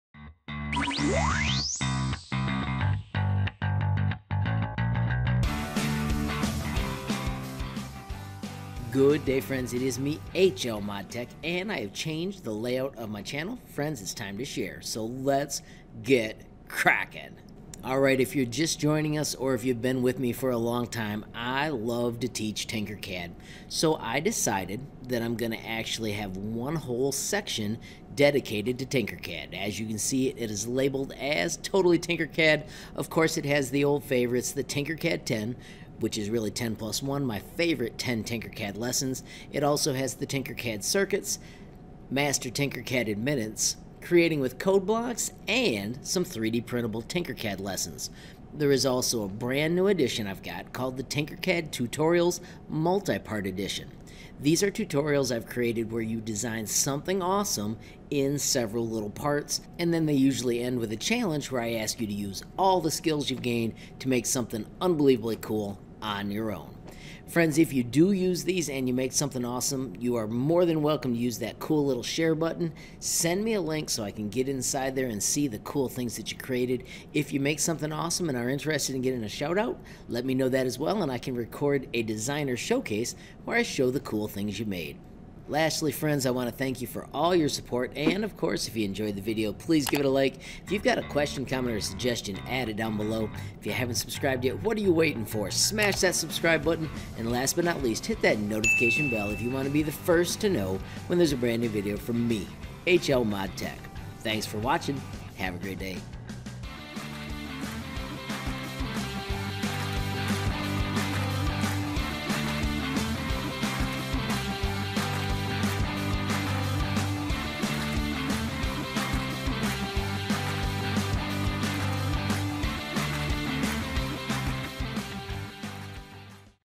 HLModTech presents Totally Tinkercad | Your One Stop Stem Shop!
Have a glorious day and keep Tinkering! I have always loved teaching with Tinkercad. Now I have the bulk of my lessons organized in their own section on the YouTube page.  My goal is to make it easier for teachers, students and life long learners to find the fun projects to solve.

We are also debuting a playlist that contains multi part Tinkercad Lessons.  The goal is for them to be great starters that end with a challenge where you create something on your own at the end of the series.